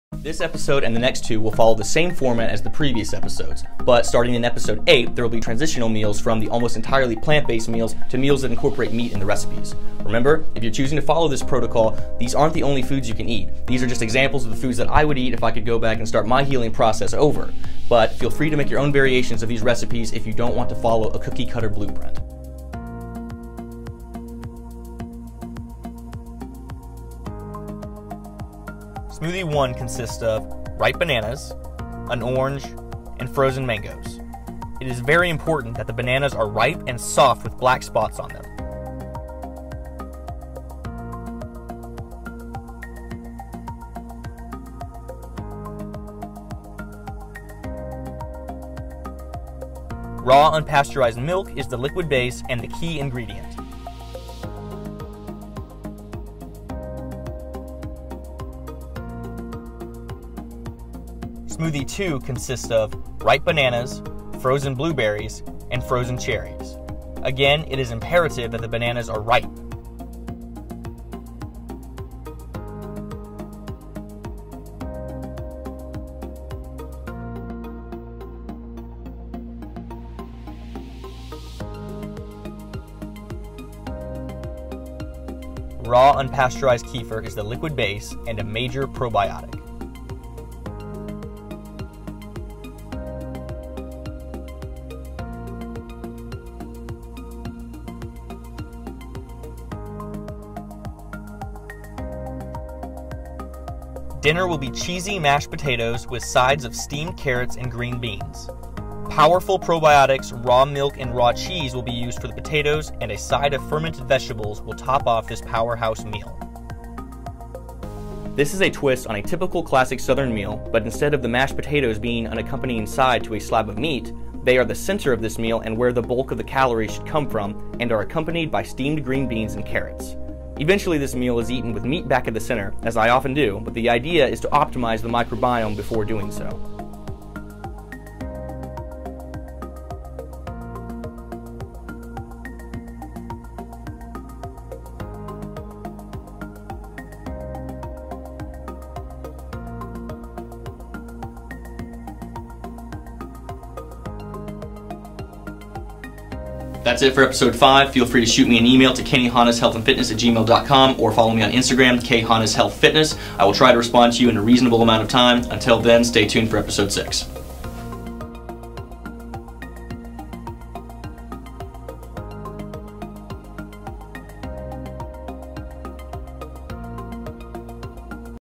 Ulcerative Colitis Boot Camp Ep. 5 | Cheesy Mashed Potatoes & 2 New Smoothies
Episode 5 in the series. Two more episodes similar to this one and then starting in episode 8 there will be transitional episodes with meat in the recipes.